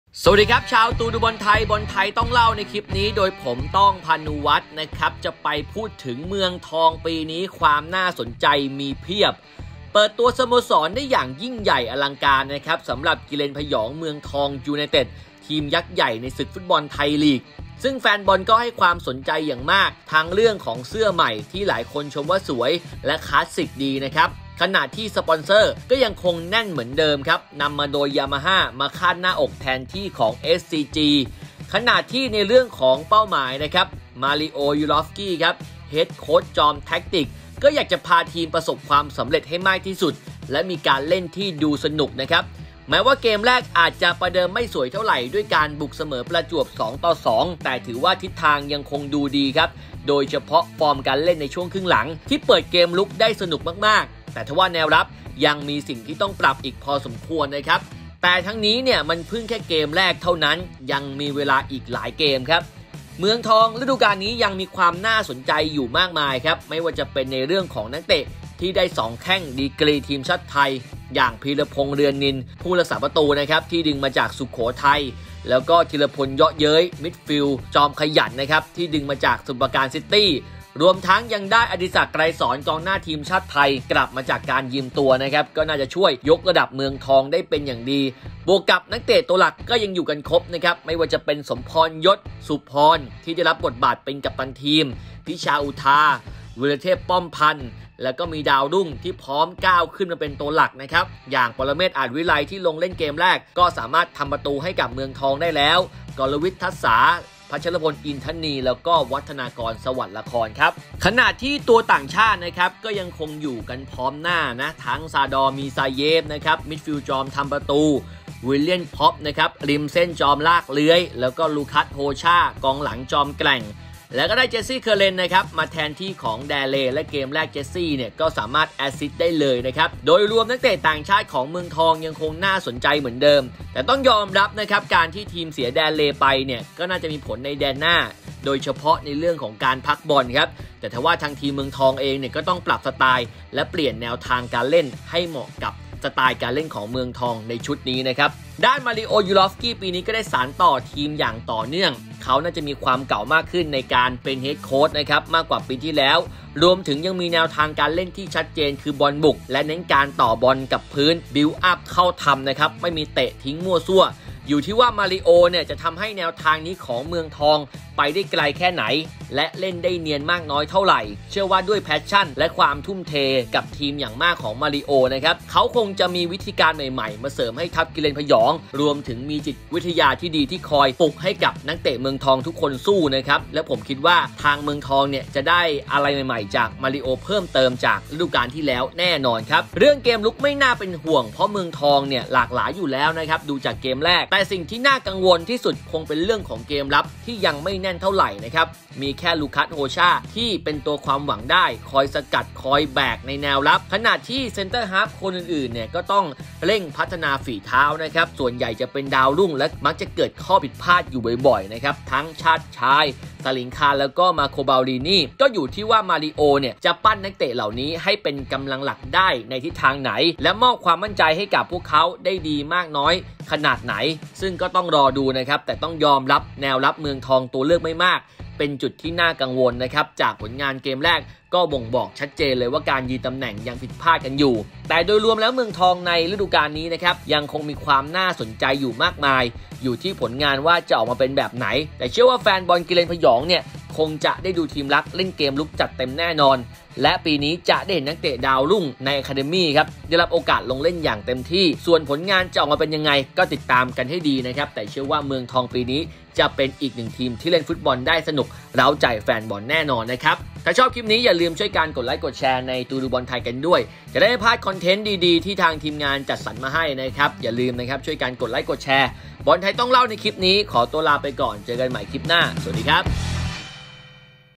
เมืองทองปีนี้ น่าสนใจมาก! | บอลไทยต้องเล่า
บอลไทยต้องเล่าวันนี้ มาพูดถึงความน่าสนใจของเมืองทอง ยูไนเต็ด หลังจากการแข่งัขันศึกรีโว่ ไทยลีก ฤดูกาล 2021 ว่ามีเรื่องอะไรบ้าง